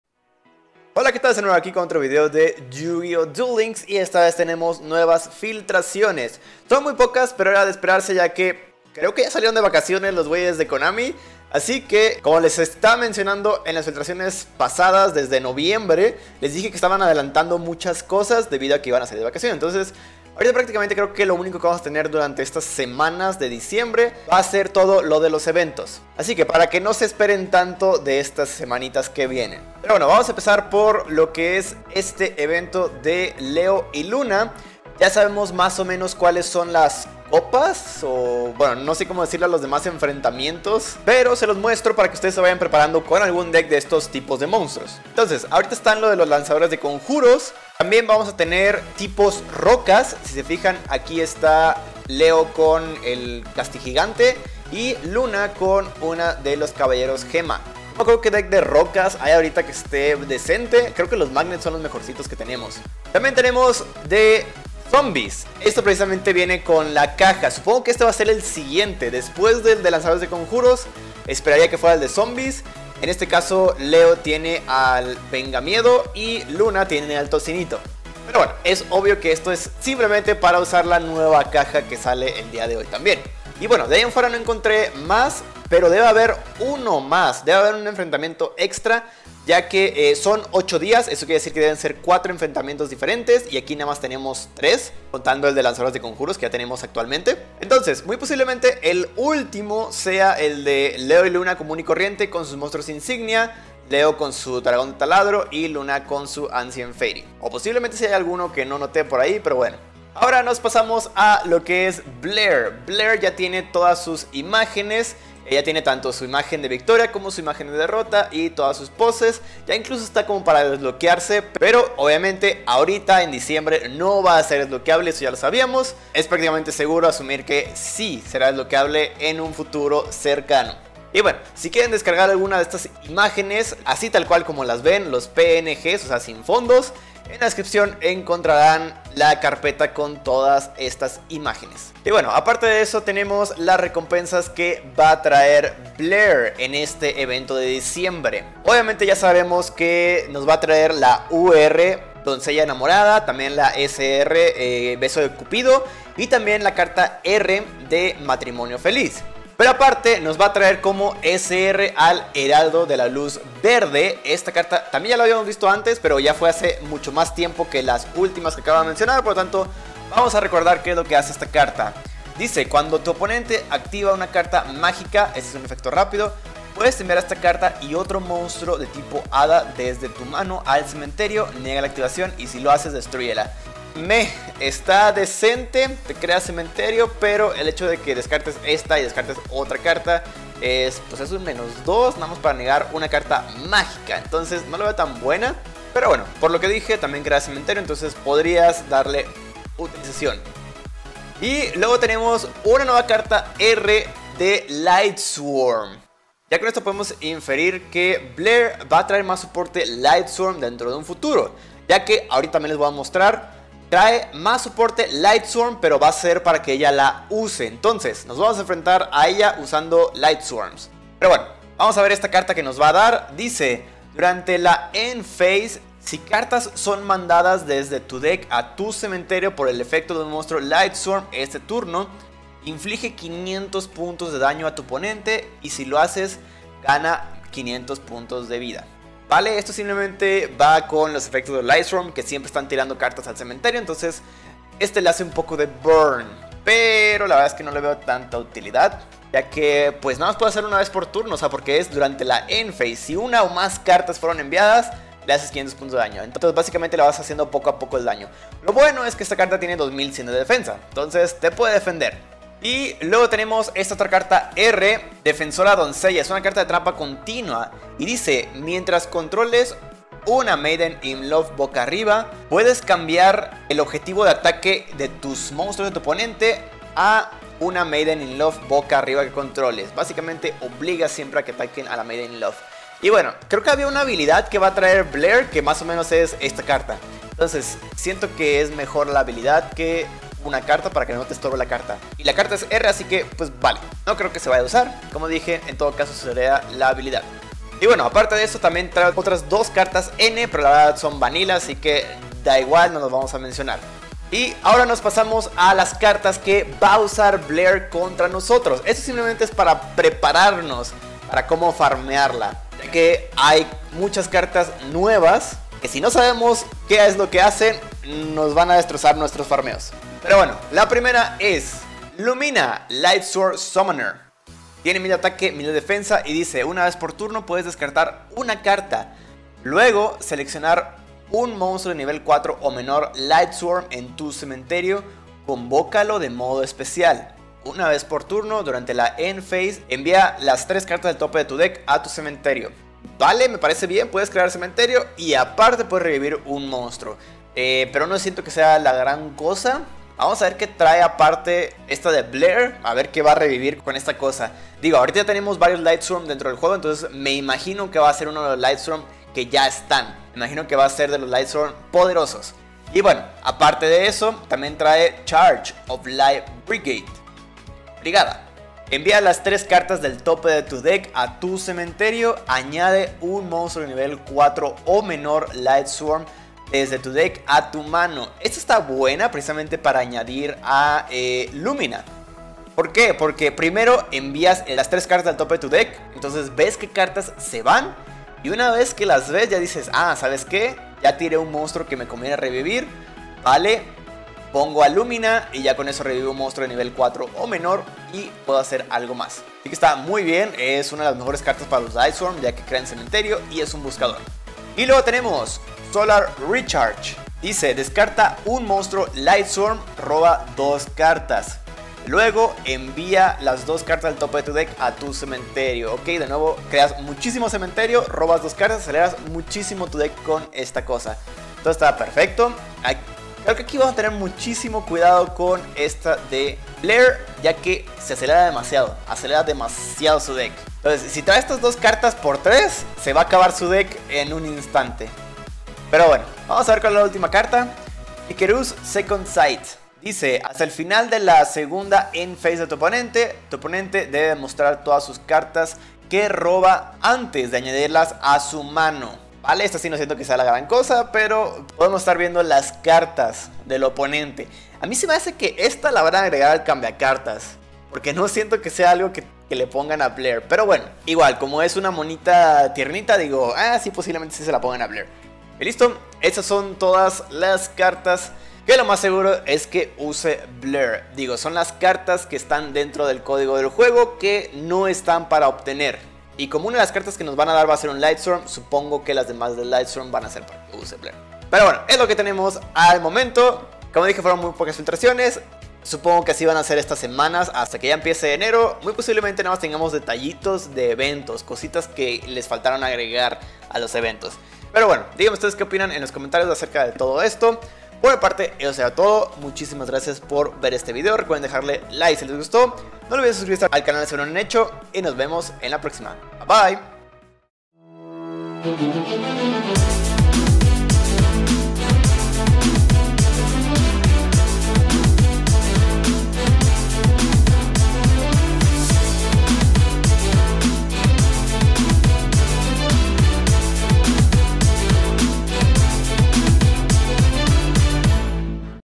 FILTRACIONES Blair Trae Lightsworn y Cartas de Anime | Yu-Gi-Oh! Duel Links
Filtraciones de la semana
-Recompensas de Blair
-Cartas que usara Blair
- Siguientes enfrentamientos carnaval